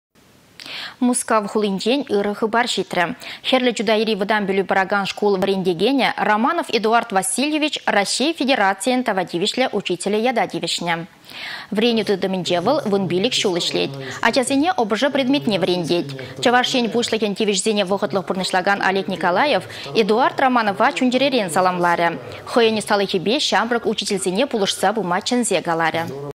Тава тивĕçлĕ учитель
Мускав хулинчен ырă хыпар çитрĕ. Хĕрлĕ Чутайри вăтам пĕлÿ паракан шкул вĕрентекене Романов Эдуард Васильевич «Раççей Федерацийĕн тава тивĕçлĕ учителĕ» ята тивĕçнĕ.